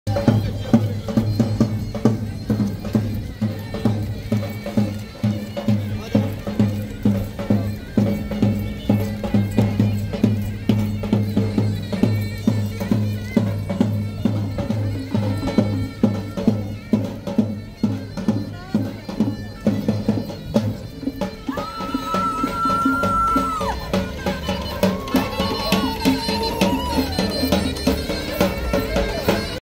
Silifke'de Bir İlk ⭐
Silifke Tarihi Taş Köprü Restorasyonu sonrası harika bir etkinliğe ev sahipliği yaptı.
Yurtiçi ve Yurtdışından gelen Folklor ekiplerinin muhteşem gösterisi sizlerle.